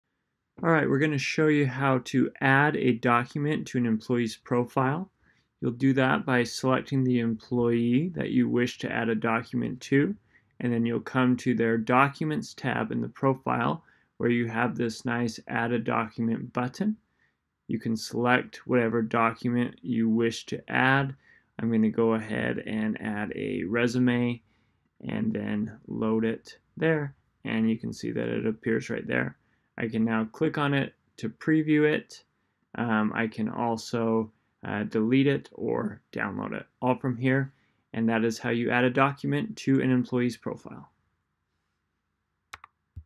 Add Document to Employee Profile | Eddy
This video shows you how to add a document to an employee's profile.